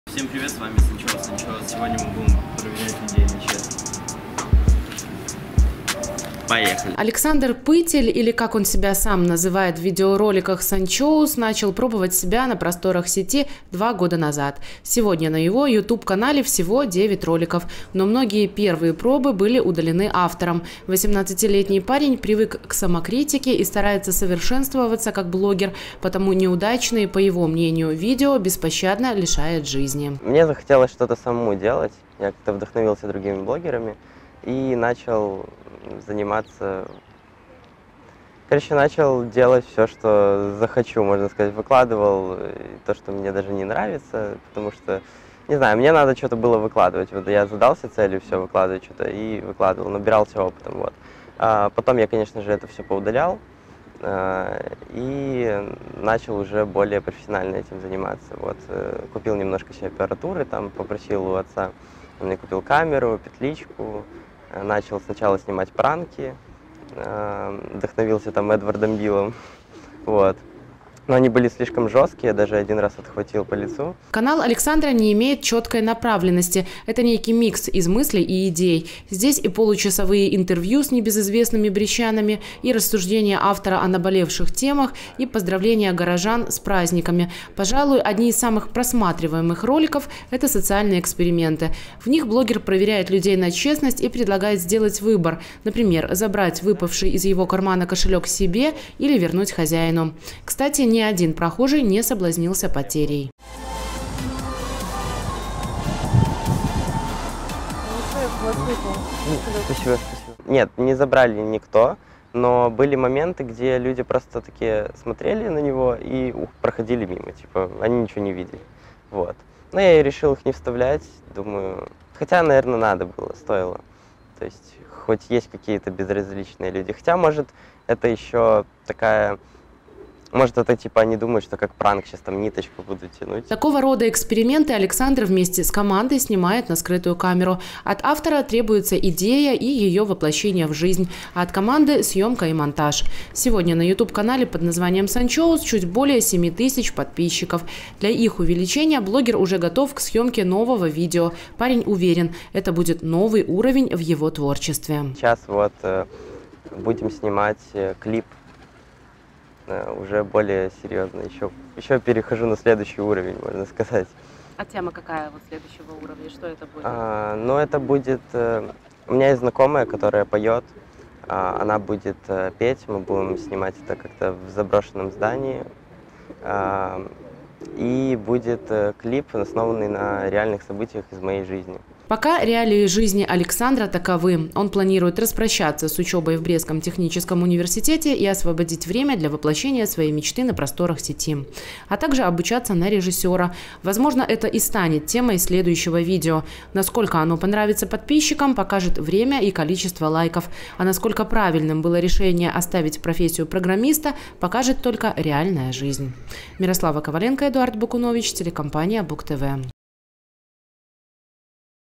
2020-04-10 г. Брест. Ютуб блогер А. Пытель. Новости на Буг-ТВ.
Журналист М. Коваленко. Оператор Э. Бакунович
Социальные эксперименты, пранки, рассуждения о личном – и всё это снято на видео, которое любой желающий может увидеть в сети. И это скорее не желание прославиться, а найти  себя в огромном мире возможностей. В следующем сюжете речь пойдёт о брестчанине Александре Пытеле, который уже не первый год стремится завоевать любовь и лайки армии поклонников. Юноша использует любые жанры видео и уже успел добиться некоторых успехов. Как свой ютуб канал может повлиять на судьбу человека – расскажет Мирослава Коваленко.